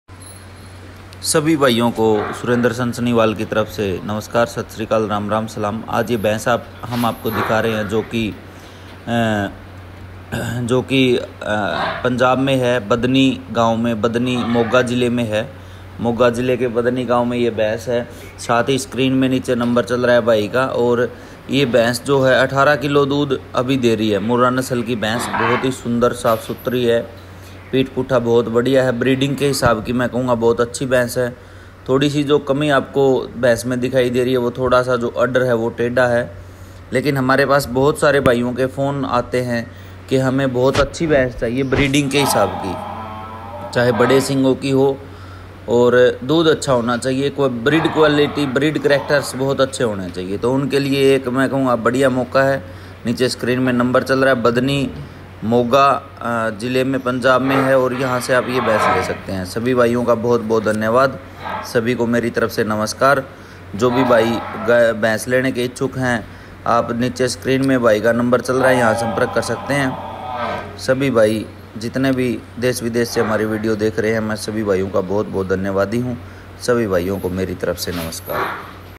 Buffalo For Sale in Moga(Punjab). Milk-18 Kg. Village- Badni
Village - Badni
District- Moga
Punjab